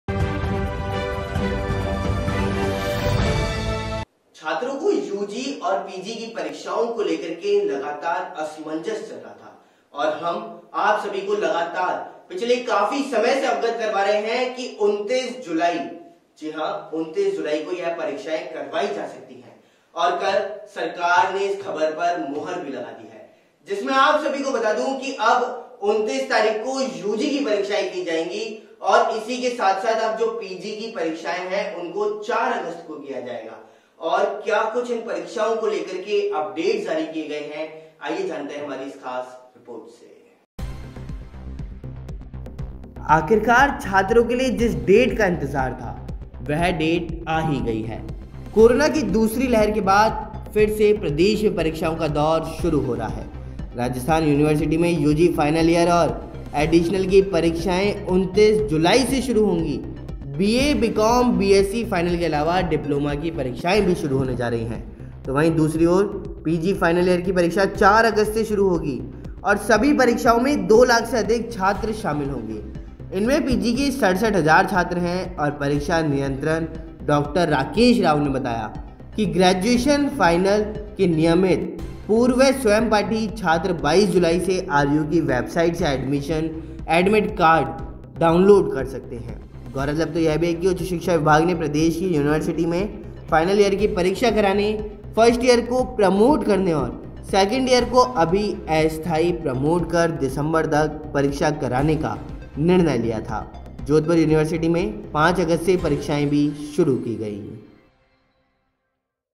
UG, PG परीक्षाओ को लेकर एक और सरकार का बड़ा फैसला
Another government's big decision regarding UG, PG examinations

UG PG PARIKSHA
RAJASTHAN UG PG PARIKSHA
UG PG PARIKSHA DATE
UG PG PARIKSHA PARMISSION LATTER

आखिरकार छात्रों के लिए जिस डेट का इंतजार था, वह आ डेट ही गई After all, the date that the students were waiting for, it is a date

aakhirakaar chhaatron ke lie jis det ka intajaar tha, vah aa det hee gaee. kaaeraaenaakaal kee doosaree lahar ke baad ab phir se pradesh mein pareekshaon ka daaair shuroo raha hai. raajasthaan yoonivarsitee mein yoojee phainal eeyar aur edeeshanal kee pareekshaen 29 julaee se shuroo hongee. beee, beekaaaim, beeesasee phainal ke alaava diplaaema kee pareekshaen bhee shuroo haaene ja rahee hain.
After all, the date that the students were waiting for, it is a date. After the second wave of Corona period, now again the period of examinations has started in the state. Rajasthan University UG final year and additional examinations will start from July 29. Apart from BA, B.Com, B.Sc final, Diploma examinations are also going to start.

आखिरकार छात्रों के लिए जिस डेट का इंतजार था, वह आ डेट ही गई। काेराेनाकाल की दूसरी लहर के बाद अब फिर से प्रदेश में परीक्षाओं का दाैर शुरू रहा है। राजस्थान यूनिवर्सिटी में यूजी फाइनल ईयर और एडीशनल की परीक्षाएं 29 जुलाई से शुरू होंगी। बीए, बीकाॅम, बीएससी फाइनल के अलावा डिप्लाेमा की परीक्षाएं भी शुरू हाेने जा रही हैं।

दूसरी ओर, पीजी फाइनल ईयर की परीक्षा 4 अगस्त से शुरू होंगी। सभी परीक्षाओं में 2 लाख से अधिक छात्र शामिल होंगे। इनमें पीजी के 67 हजार छात्र हैं। परीक्षा नियंत्रक डाॅ. राकेश राव ने बताया कि ग्रेजुएशन फाइनल (पास व ऑनर्स) के नियमित, पूर्व व स्वयंपाठी छात्र 22 जुलाई से आरयू की वेबसाइट से एडमिट कार्ड डाउनलाेड कर सकते हैं।

गाैरतलब है कि उच्च शिक्षा विभाग ने प्रदेश की यूनिवर्सिटी में फाइनल ईयर की परीक्षा कराने, फर्स्ट ईयर काे प्रमाेट करने और सैकंड ईयर काे अभी अस्थायी प्रमाेट कर दिसंबर तक परीक्षा करने का निर्णय लिया था। जाेधपुर यूनिवर्सिटी में 5 अगस्त से परीक्षाएं शुरू हाेगी।